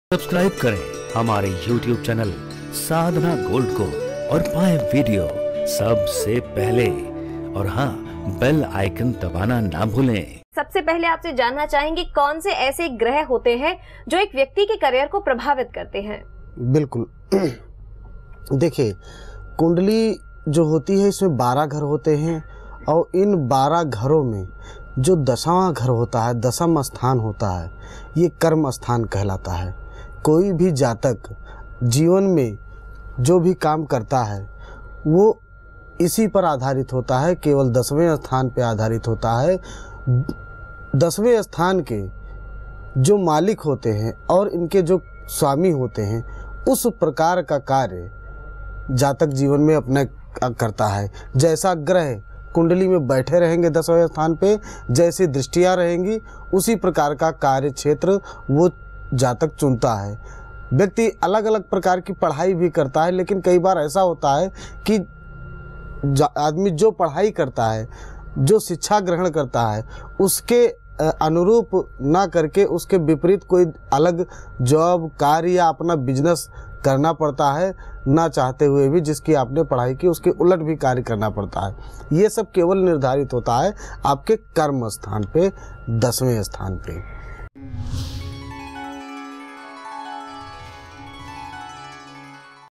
हमारा करियर इन ग्रहों से होता है प्रभावित। आचार्य संदीप शास्त्री जी। Sadhna Astro
Thanks.